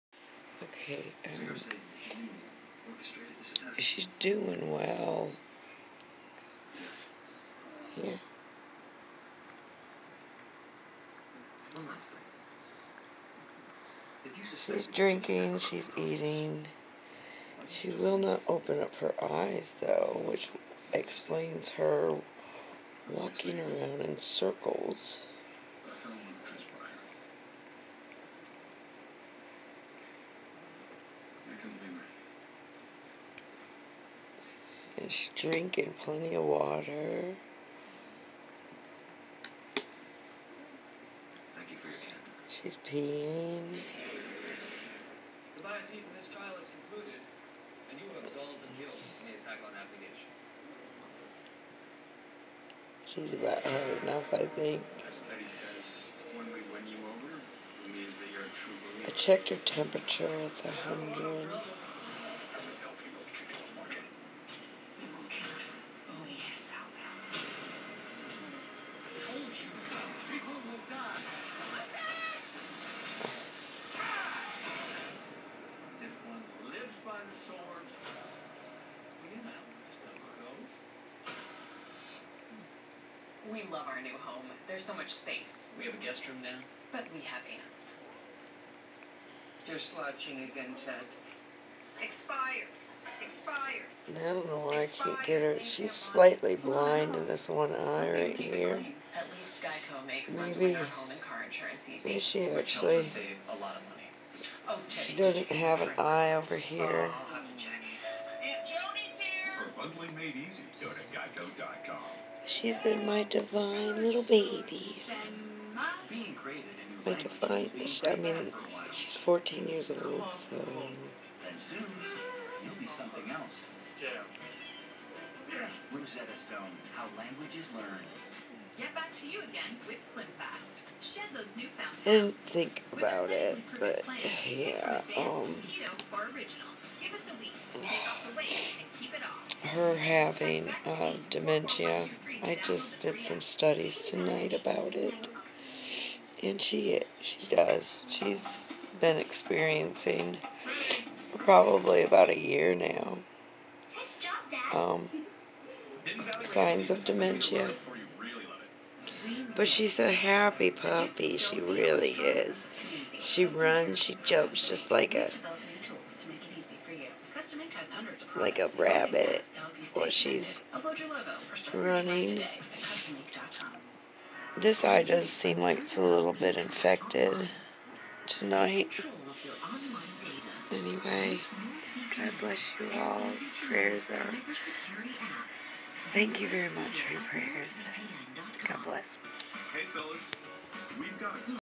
Prayers for Ruby 🙏 Up date 2 day 4/2021
2 years ago my fourteen-year-old Ruby became blind in one eye completely. two years later the same things happening to her good eye. I realize she has dementia and gone completely blind